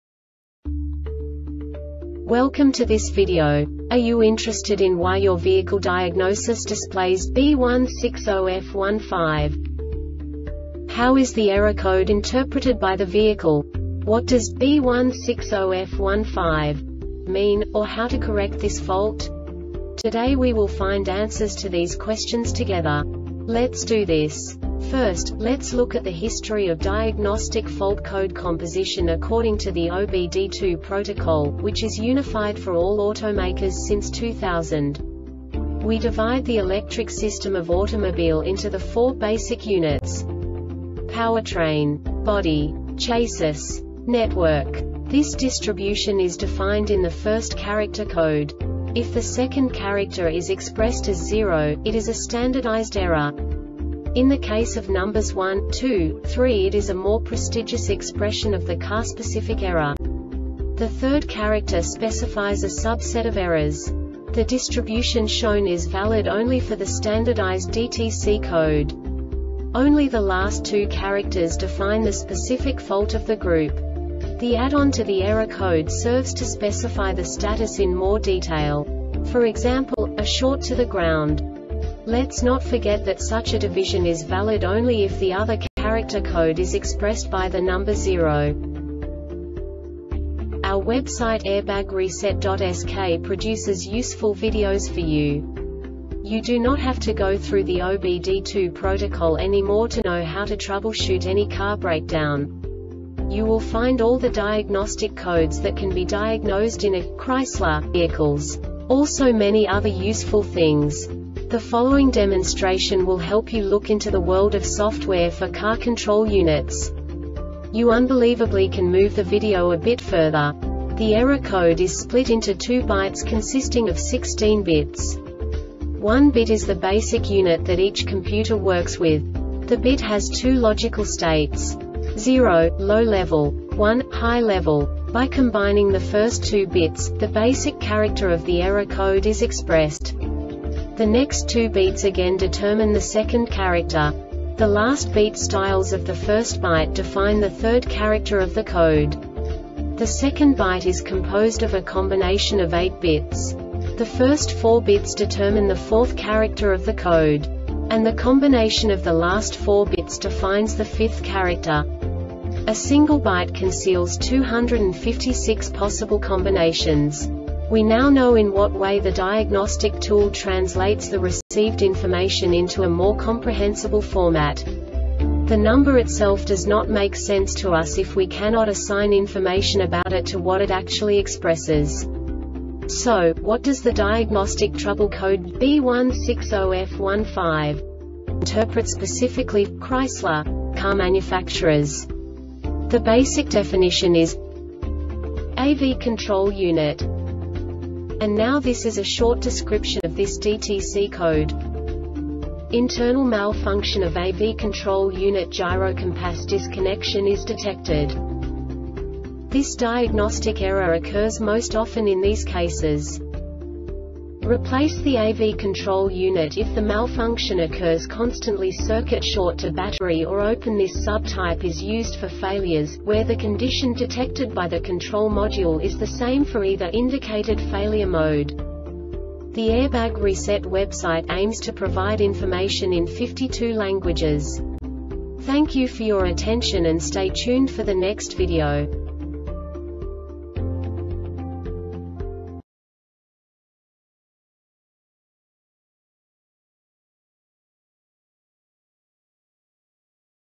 DTC Chrysler B160F-15 Short Explanation
Contents:
0:21 Basic DTC analysis according to OBD2 protocol standard.
1:48 Insight into programming
2:58 A diagnostically understandable description of the Diagnostic Trouble Code
3:05 Basic error definition

Make:
Chrysler
Code:
B160F 15
Definition:
AV Control Unit
Description:
Internal malfunction of AV control unit (gyrocompass disconnection) is detected.